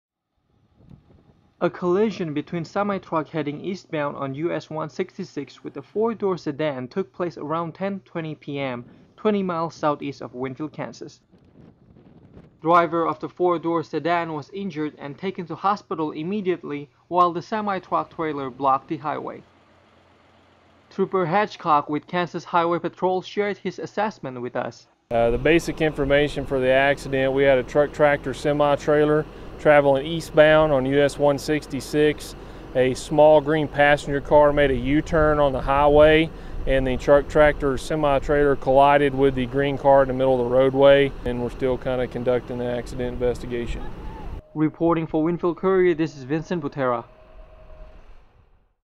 Late Night Accident on US166
A collision between semi-truck heading eastbound on US166 with a 4 door sedan took place around 10.20pm ---   20 miles southeast of Winfield, KS